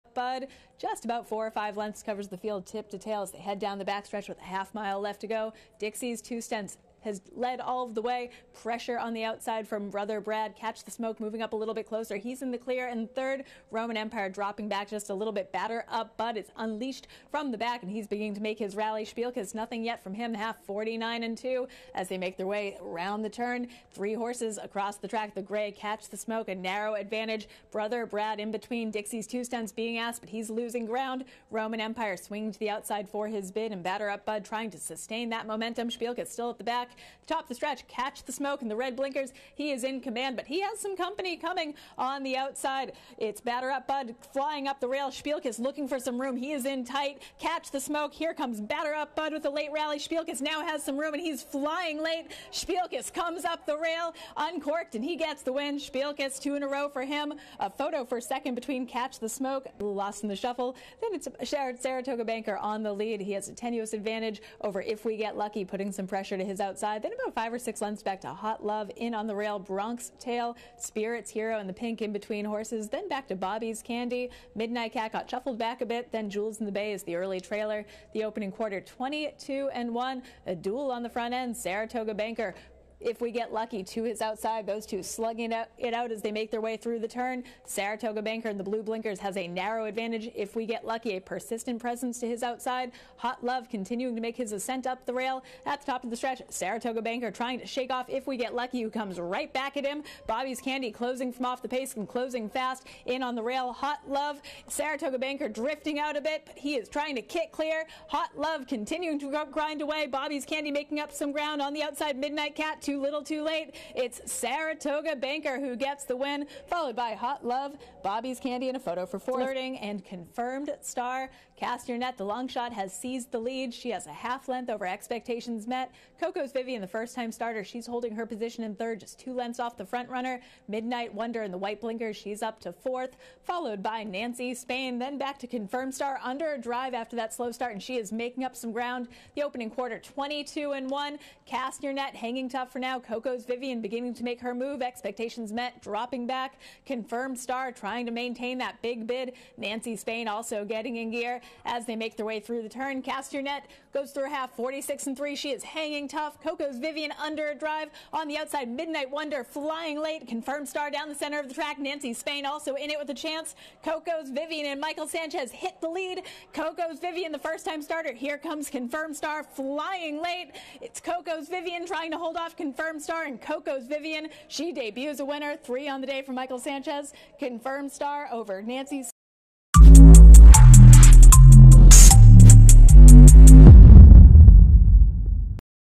4ª., 5ª. y 7ª. CARRERA - SHPILKES, SARATOGA BANKER y COCOA'S VIVIAN con Mychel Sánchez
SON SEIS
Así ganó "El Pistolero" Mychel Sánchez la 4ª., 5ª. Y 7ª. Carrera de hoy miércoles 18/10 en Parx Racing con los ejemplares #5 SHPILKES desde el último y por dentro pasó de viaje, el #4 SARATOGA BANKER en punta y finalmente con el #9 COCOA'S VIVIAN desde la partida.

4ª. Carrera
Lote: Mile 70 Yards | Open | 3 Year Olds And Up | STARTER ALLOWANCE | PURSE: $32,250

Pizarra:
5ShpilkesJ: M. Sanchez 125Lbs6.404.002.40
7Batter Up BudJ: A. Castillo 122Lbs 9.003.80
4Catch the SmokeJ: J. Torres 122Lbs  2.60
Times in 5ths: :241   :492   1:144   1:42   1:461
5ª. Carrera
Lote: Furlongs | Open | 3 Year Olds And Up | STARTER OPTIONAL CLAIMING : $16,000 | PURSE: $25,750

Pizarra:
4Saratoga BankerJ: M. Sanchez 118Lbs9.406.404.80
8Hot LoveJ: R. Mitchell 118Lbs 7.804.80
1Bobby's CandyJ: J. Laprida 122Lbs  7.60
Times in 5ths: :221   :454   :584   1:131
7ª. Carrera
Lote: Furlongs | Fillies and Mares | 3 Year Olds And Up | MAIDEN SPECIAL WEIGHT | PURSE: $67,000

Pizarra:
9Cocoa's VivianJ: M. Sanchez 120Lbs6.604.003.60
6Confirmed StarJ: F. Pennington 120Lbs 4.403.60
1Midnight WonderJ: S. Gonzalez 120Lbs  13.40
Times in 5ths: :221   :463   :593   1:132